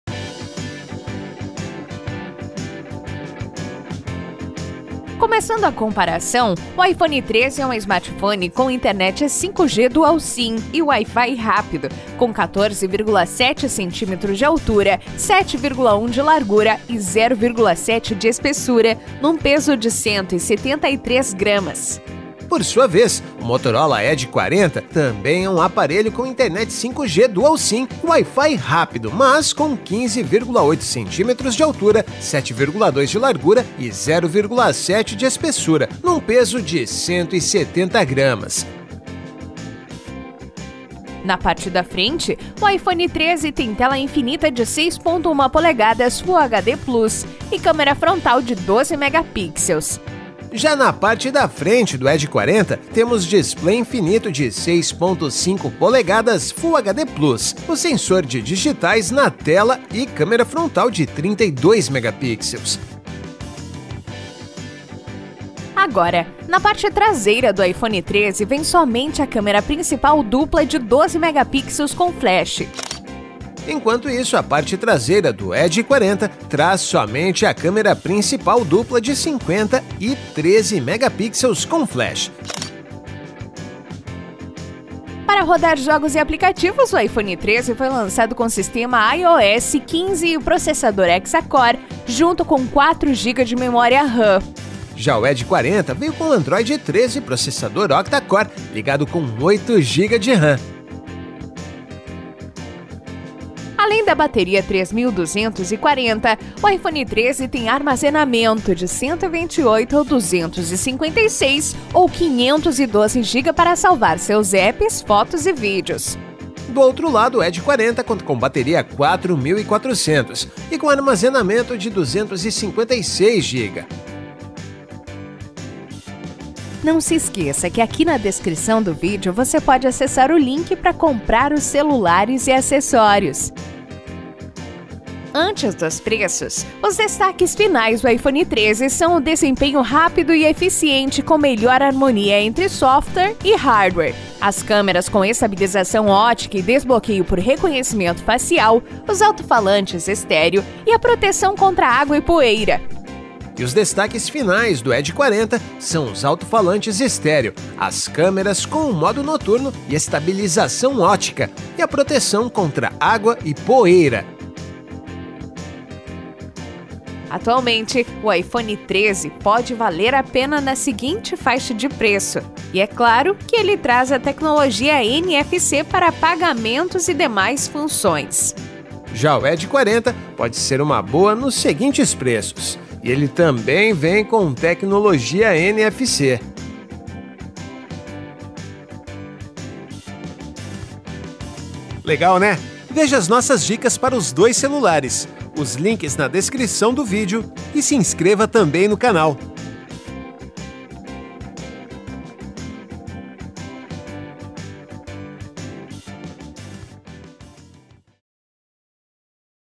iPhone 13 vs Motorola Edge 40 (Comparativo & Preços)
iPhone 13 normal vs Motorola Edge 40 normal. Confira a ficha técnica do Apple iPhone 13 e do Motorola Edge 40, uma comparação de tamanho exclusiva (3D), e muito mais.

(iPhone 13)

Pergunta:
Você compraria o Moto Edge 40? A partir de que preço?